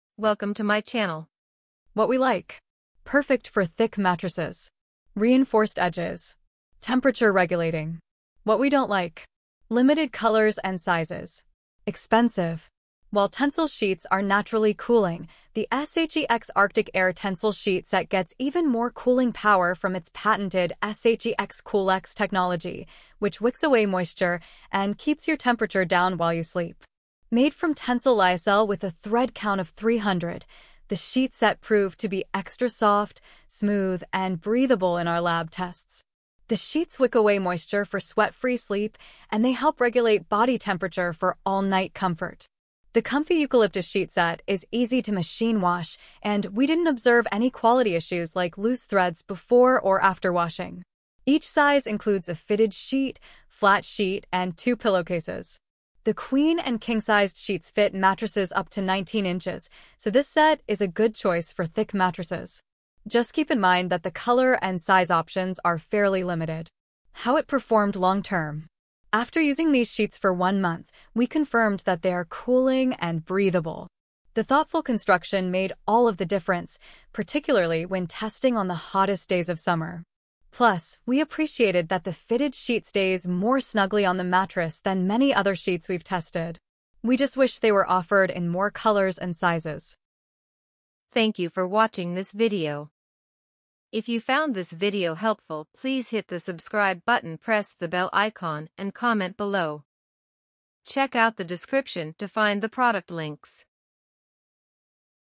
Sheex Arctic Aire Max Sheet Set Review
in this video we will show Sheex Arctic Aire Max Sheet Set Review

in this Sheex Arctic Aire Max Sheet Set, I cover everything from the beautiful appearance to the functionally of the item.